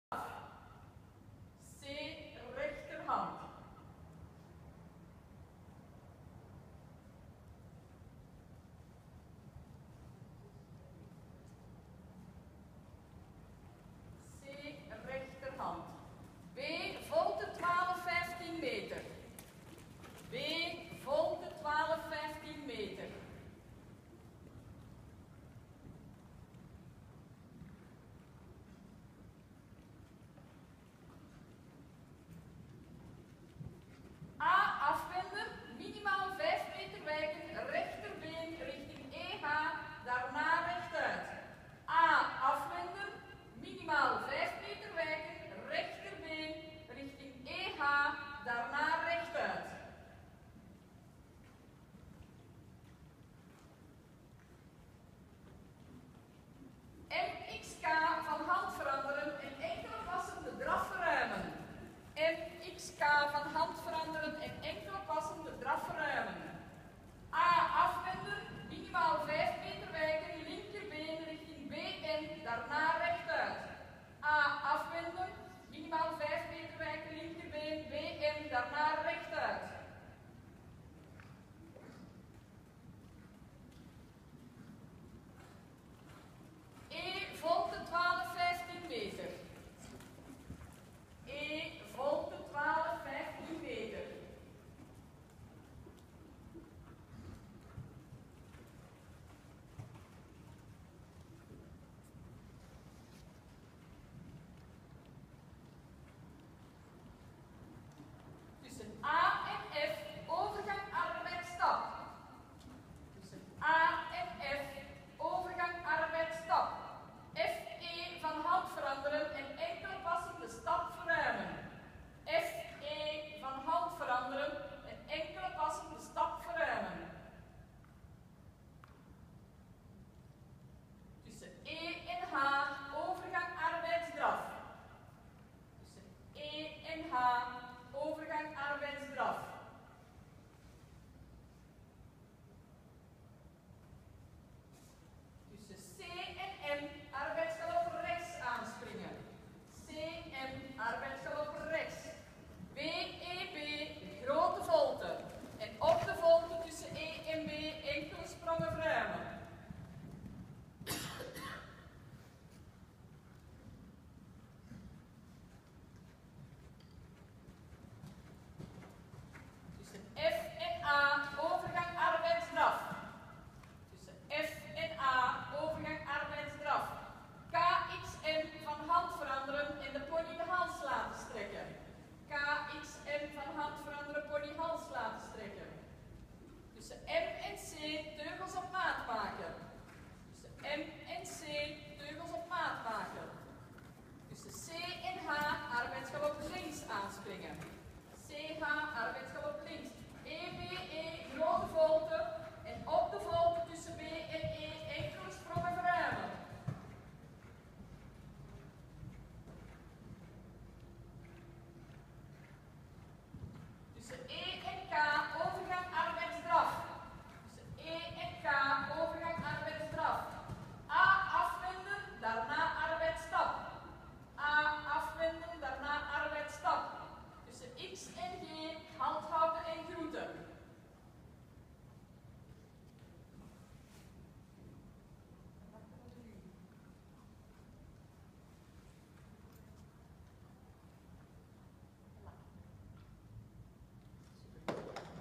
Megan van Vugt - Tilburg 05-11-2016 - Dressuur Pony D L1
Megan en Bianco tijdens selectie wedstrijd in Tilburg.